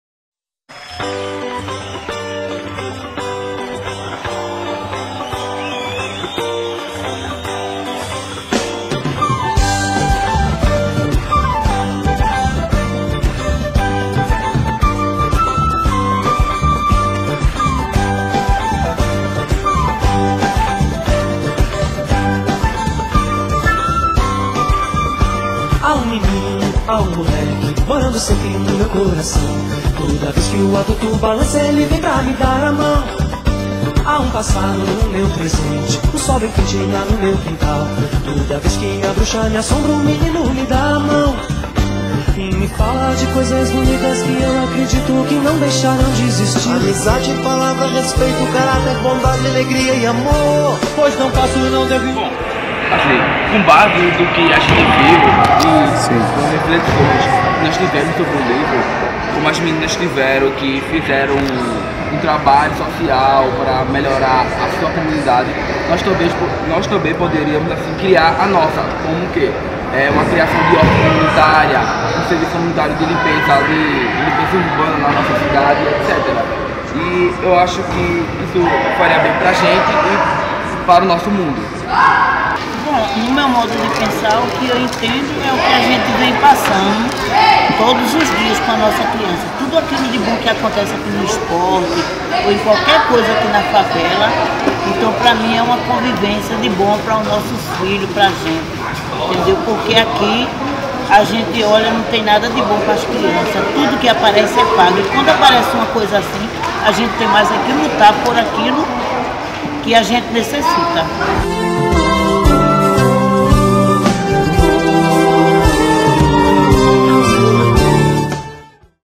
Atividades do Serviço de Convivência e Fortalecimento de Vínculos da Cidade de Recife Localização S
Atividades do Serviço de Convivência e Fortalecimento de Vínculos da Cidade de Recife
Localização: Sport Clube do Recife

Equipe: Adriana Silva Bárbara Melo Lenaida Teles Marina Paula Nunes Raissa Elis Saint-Clair Teo Armando Thiago Moura
SCFV-Sport